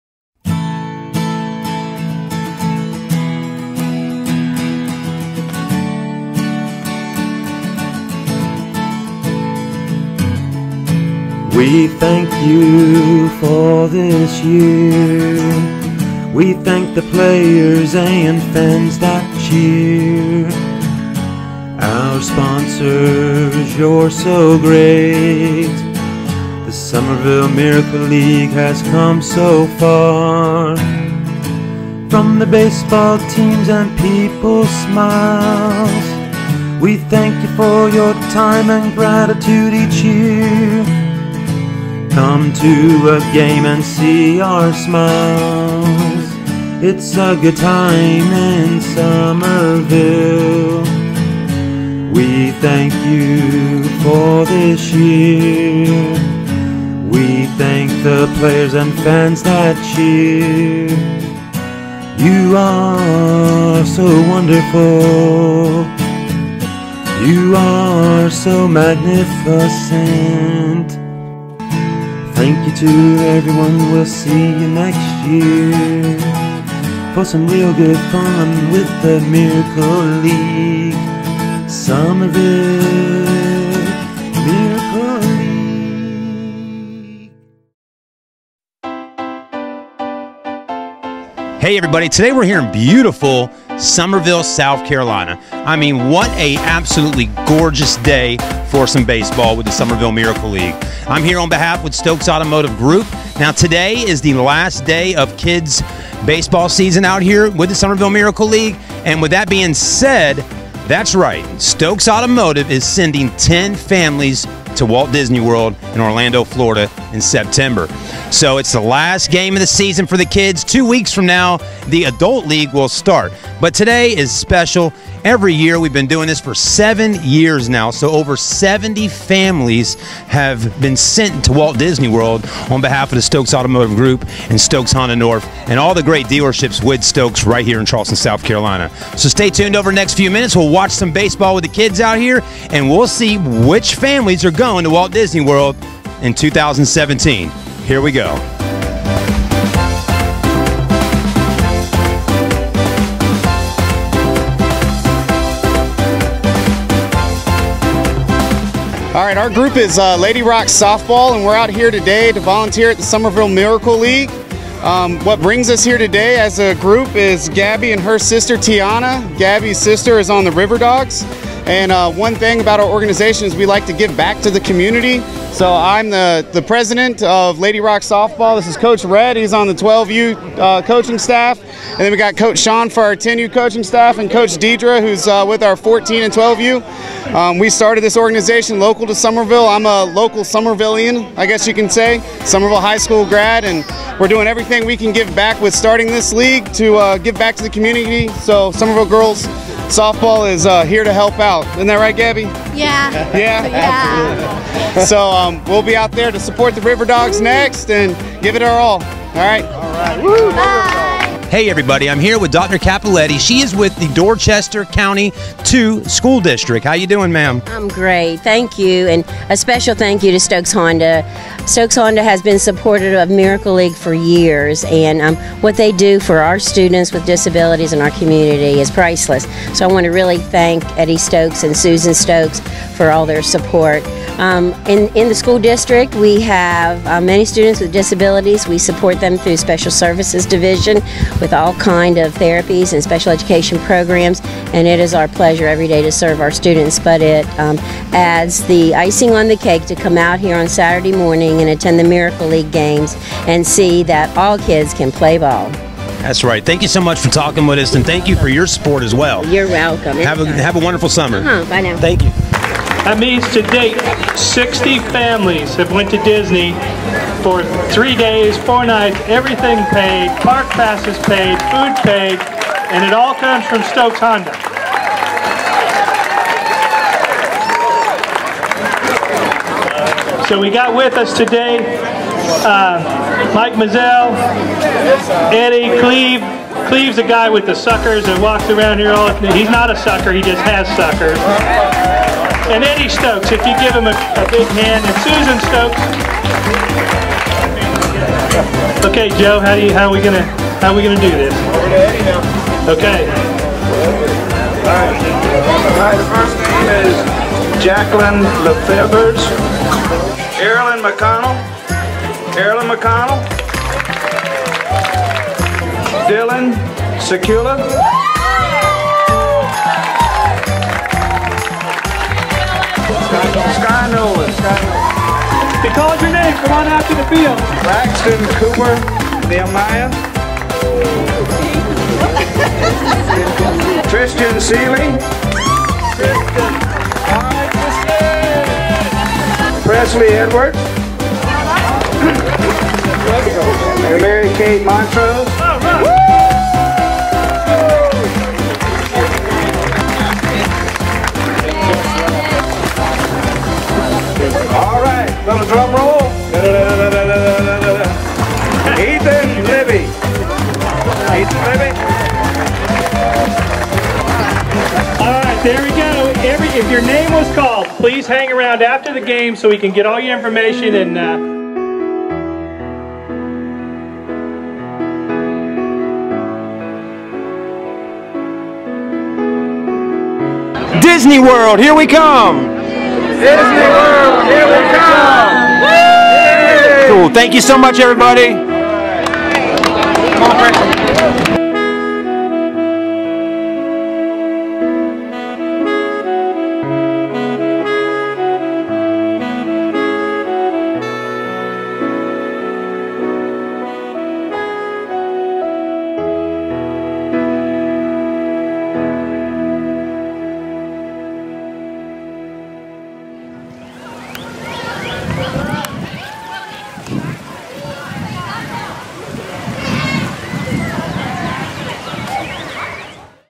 Summerville Miracle League 2017 - Stokes Volkswagen - North Charleston, SC
Here's a wonderful video from the last kids game of the season in 2017. Stokes Automotive sends 10 Families each year to Walt Disney World. This year will be our 7th year doing this.

Song in beginning of Video written & sung by: Chad Dolbier